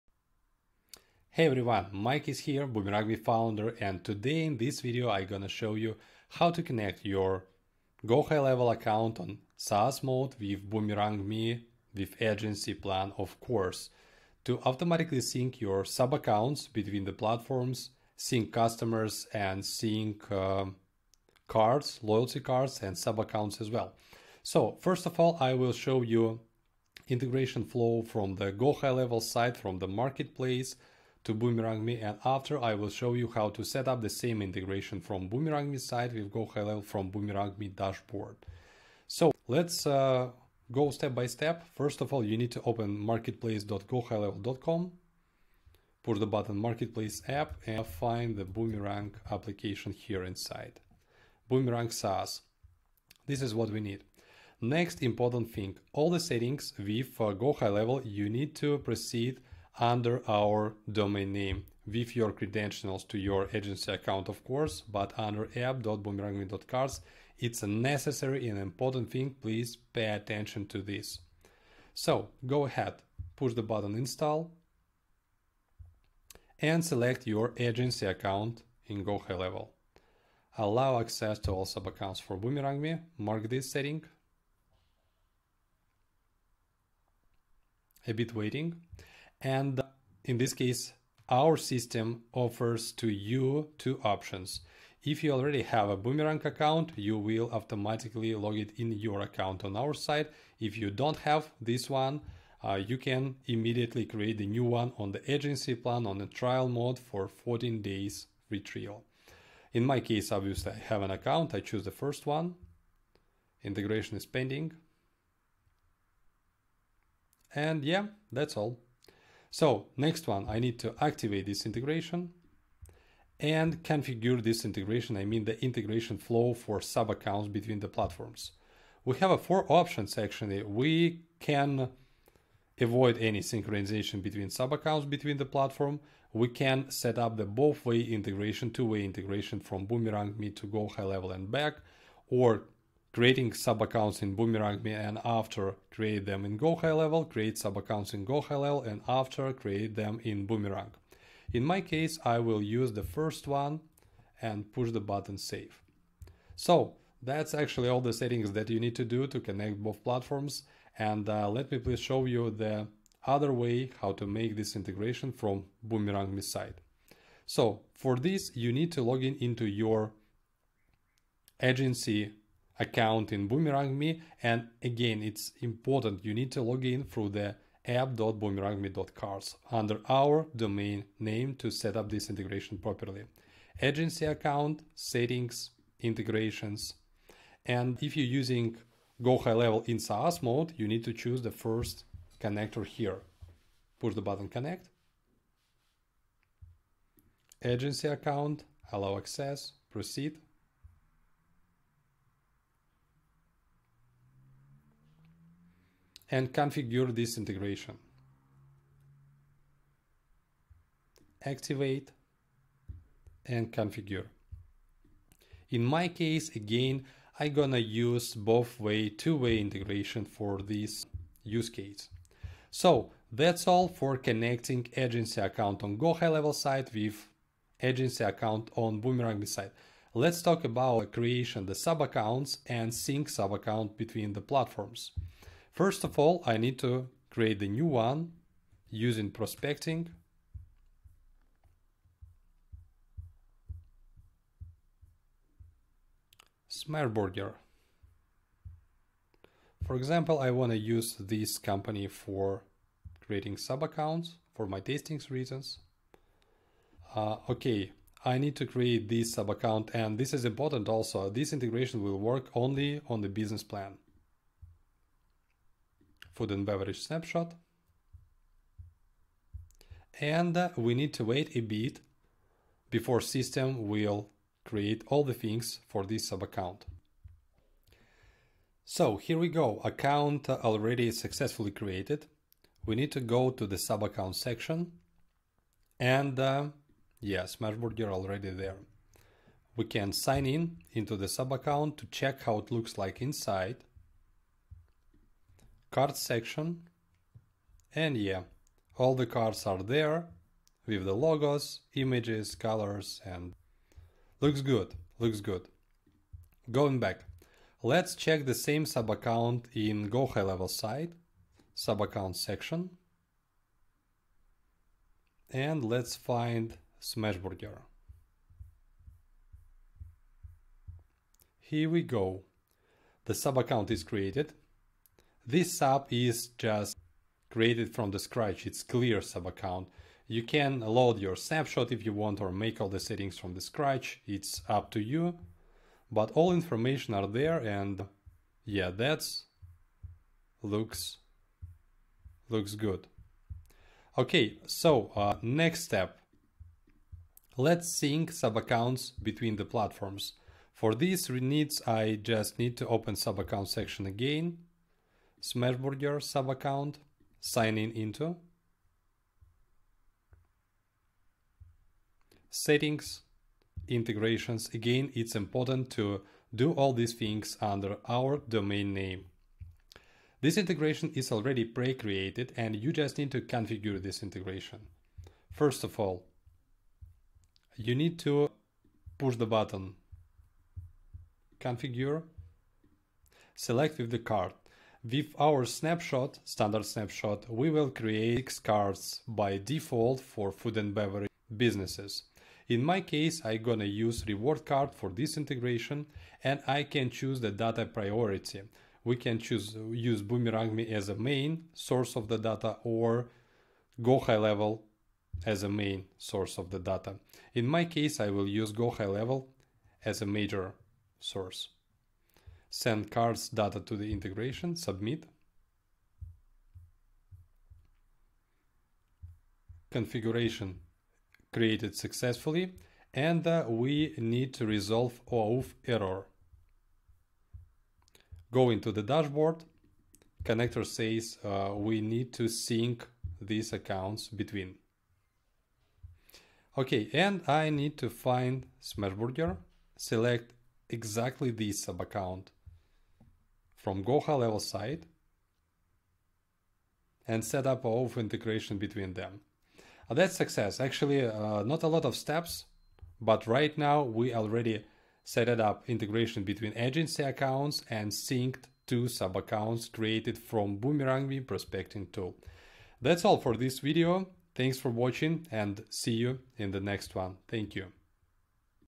Connecting LC SaaS Account with Boomerang integration 2.0 🔗
In this video, I, Mike, the founder of Boomerang, will guide you through the process of connecting your LeadConnector account on SaaS mode with Boomerang. I will show you the integration flow from the LeadConnector side and from the Boomerang dashboard. I will also explain the necessary settings and options for syncing sub-accounts between the platforms. No action is required from you, but this video will provide important information for setting up the integration.